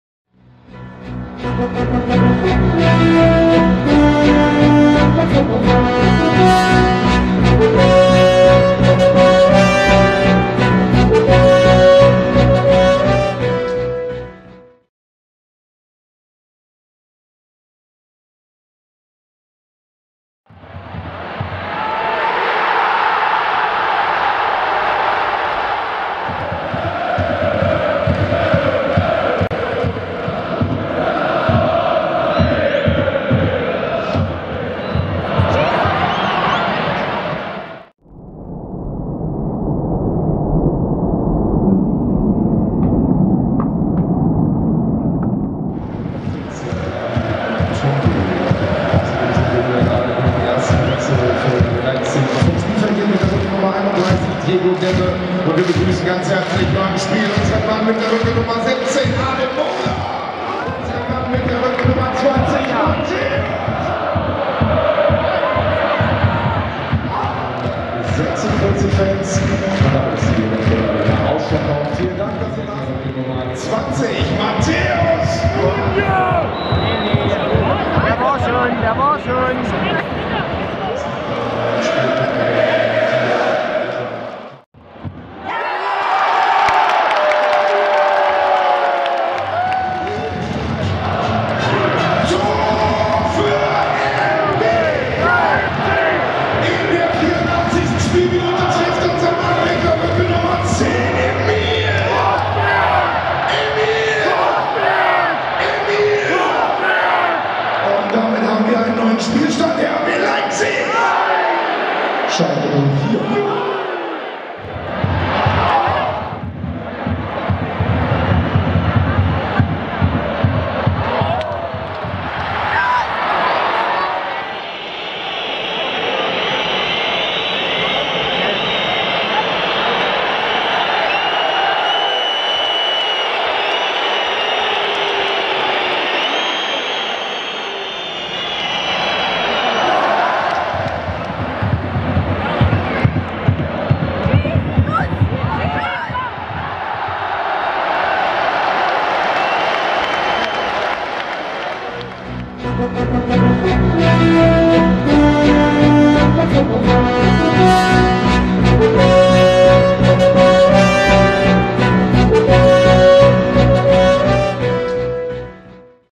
RB Leipzig vs FC Schalke 04 1:3 Highlights Tore Malocher schlagen Rote Bullen
Ein Nachmittag zum Vergessen! Und ein viel zu spätes Anschlusstor. Schnell abhaken. Weil: Die Franzosen kommen! Championsleague steht an. Da wollen wir ins Achtelfinale ...

auch auf Facebook und Instagram